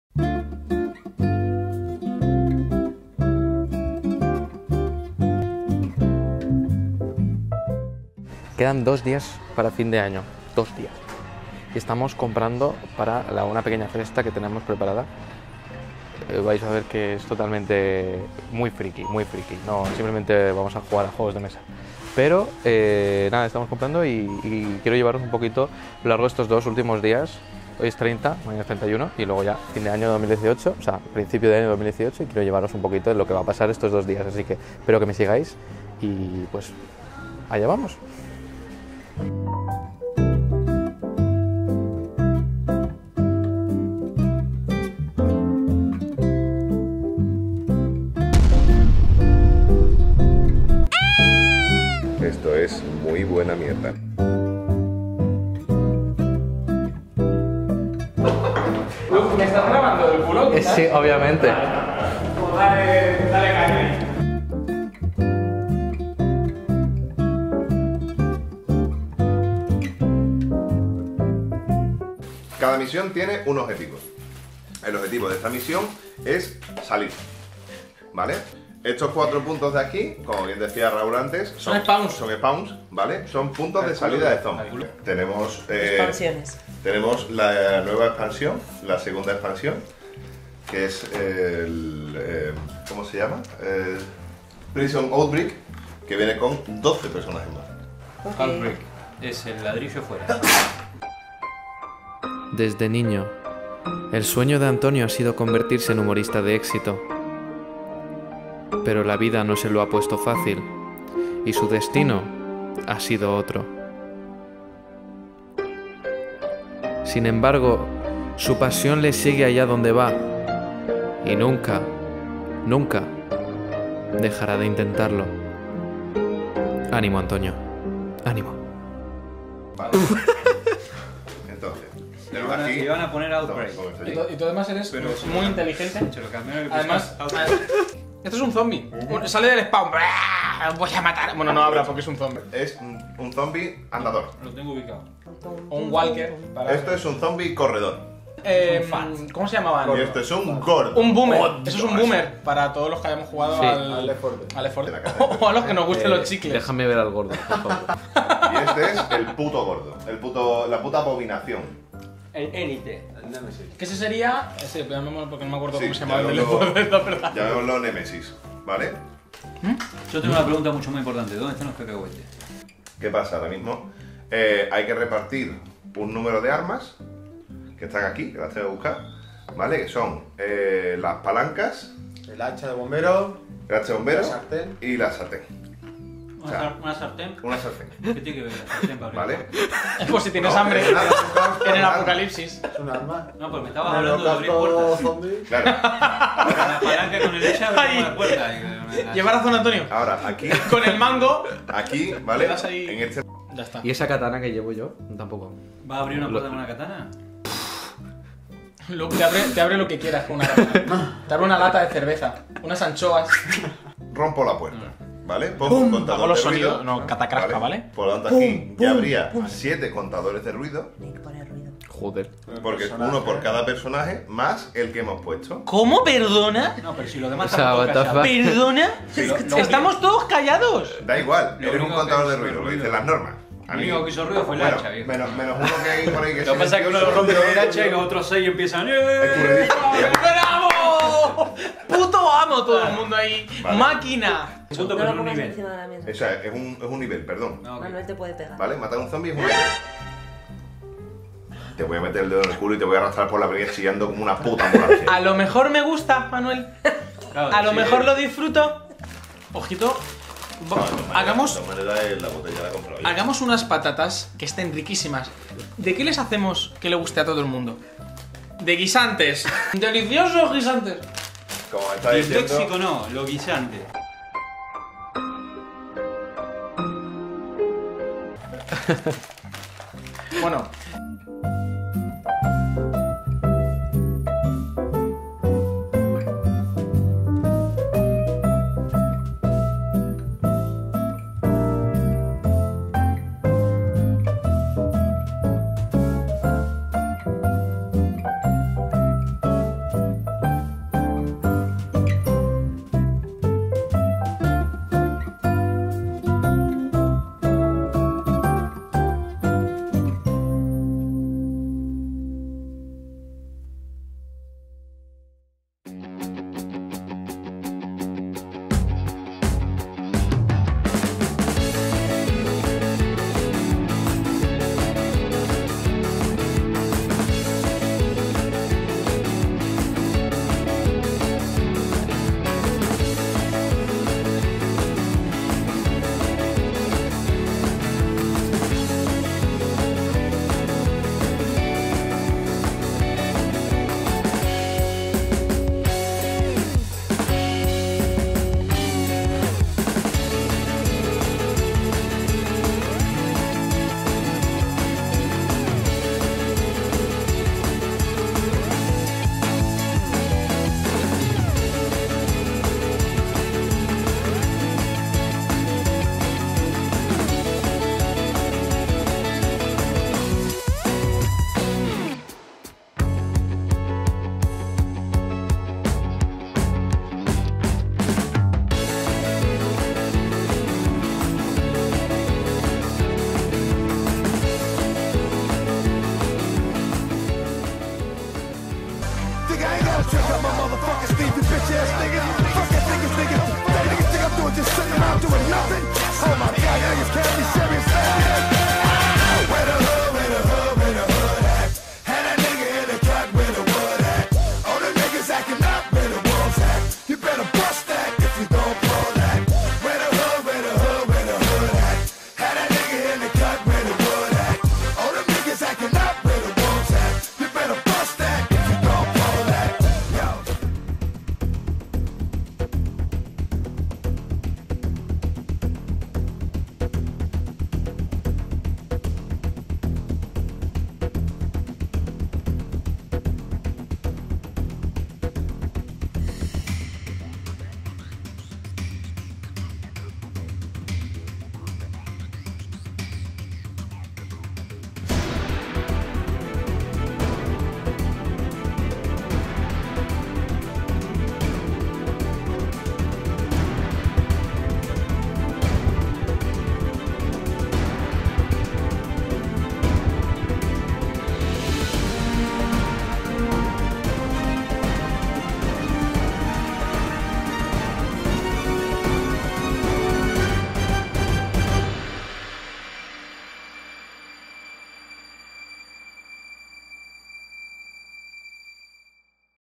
HUMOR, GUISANTES Y ZOMBIS | NOCHEVIEJA 2018
Los últimos dos días del año hay que disfrutarlos, por lo que en este video se junta todo lo que más nos gusta: humor, guisantes, zombis... ¿Qué más se puede pedir?
Y si es junto a una compañía 10/10 mejor aún.